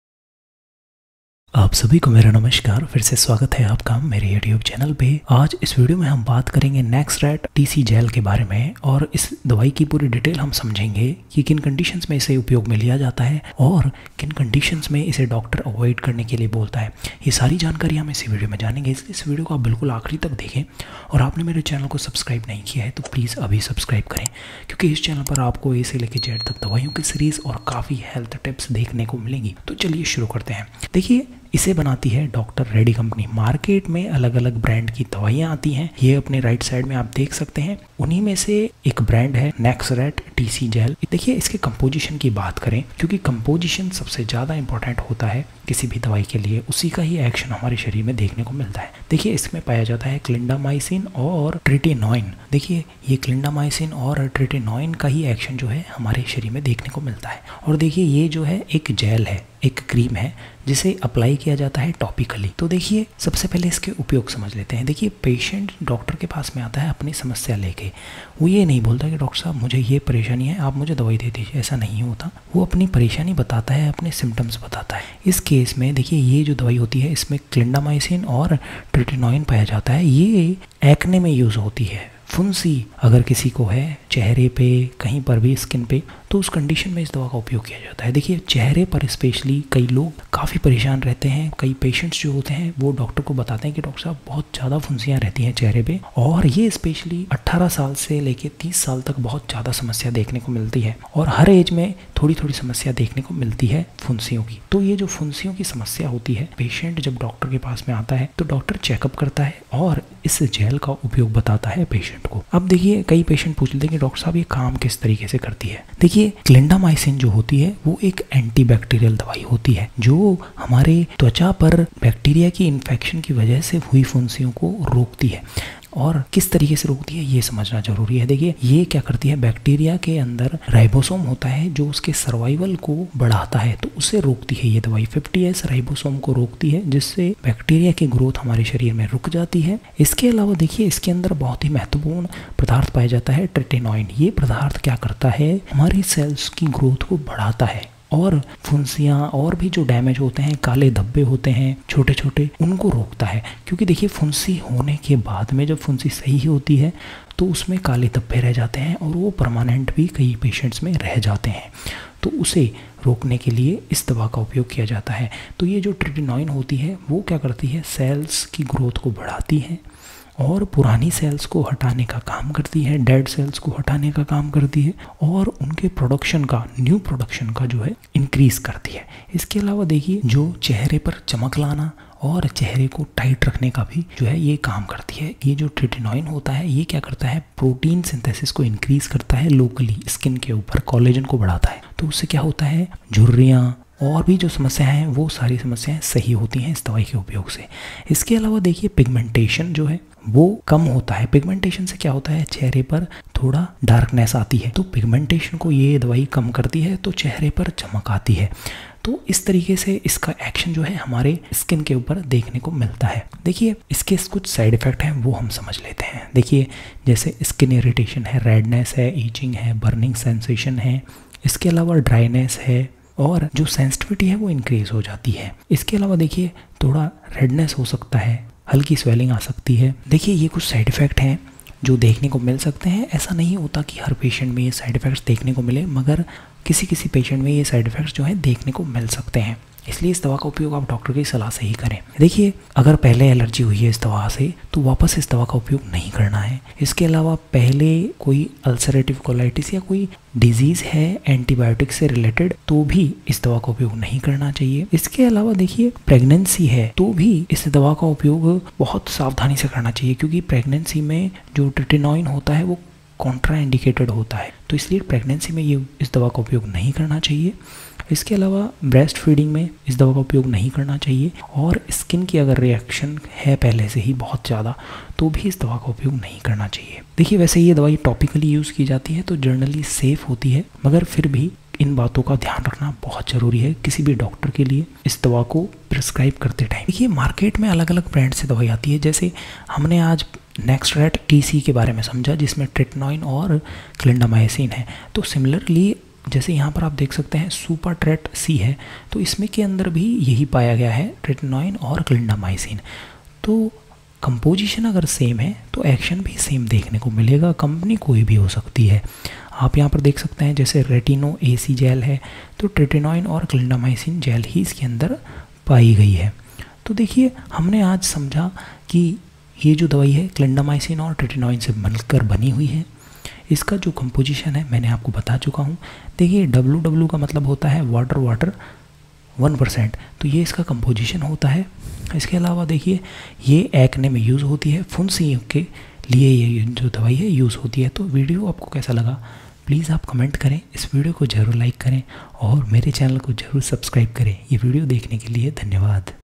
नेक्सरेट TC जेल (Clindamycin & Tretinoin) जेल मुहांसों और त्वचा की देखभाल के लिए in Hindi
आज के इस वीडियो में हम बात करेंगे नेक्सरेट-TC जेल के बारे में। नेक्सरेट-TC जेल क्या है, इसके उपयोग, फायदे, और इसे इस्तेमाल करने के सही तरीके के बारे में विस्तार से जानेंगे।

नेक्सरेट-TC जेल क्या है?
नेक्सरेट-TC जेल एक औषधीय जेल है जिसे मुख्य रूप से त्वचा की समस्याओं के इलाज के लिए इस्तेमाल किया जाता है। इसमें टेरबिनाफाइन और क्लोट्रिमेजोल जैसे सक्रिय तत्व होते हैं, जो त्वचा के संक्रमणों को ठीक करने में मदद करते हैं।

नेक्सरेट-TC जेल के उपयोग:

त्वचा के फंगल संक्रमण का इलाज
एथलीट्स फुट (पैरों के फंगल संक्रमण) का उपचार
दाद (रिंगवर्म) का उपचार
खुजली और त्वचा की जलन को कम करना